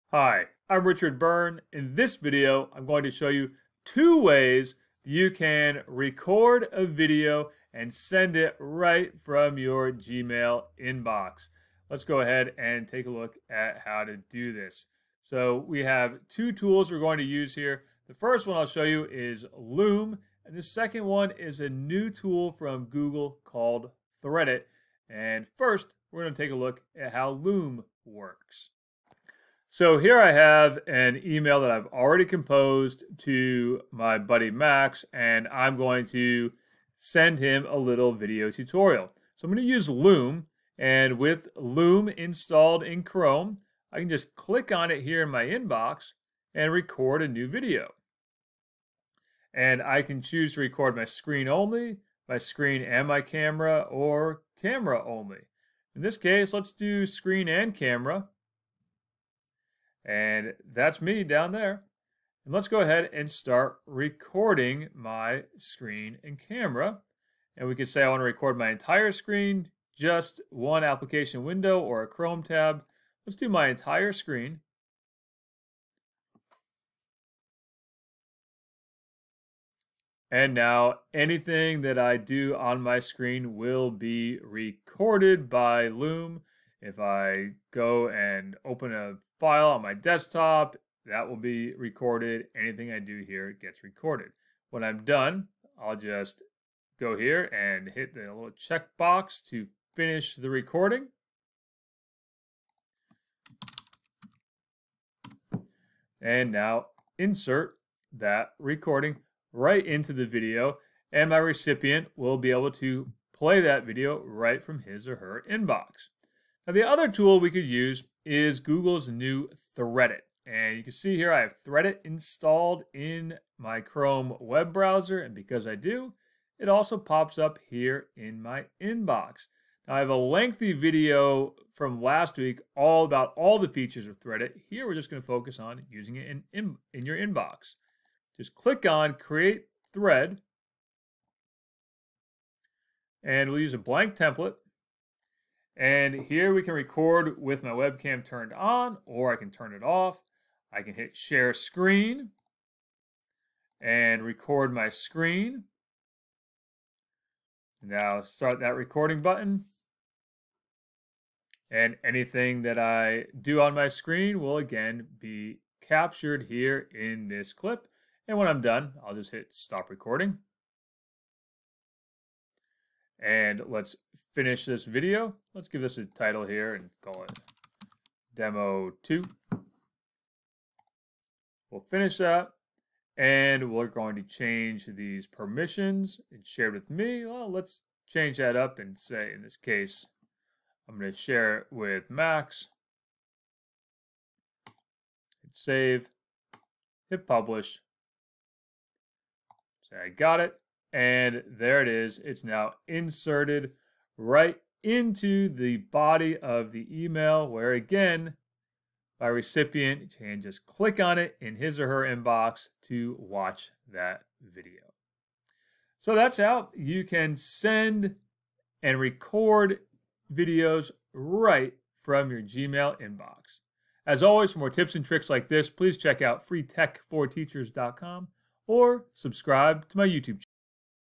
Two Ways to Create Videos in Your Gmail Inbox
Watch this video to learn two ways to create screencast videos directly from your Gmail inbox. This is a great way to provide tech support without having to write out step-by-step directions.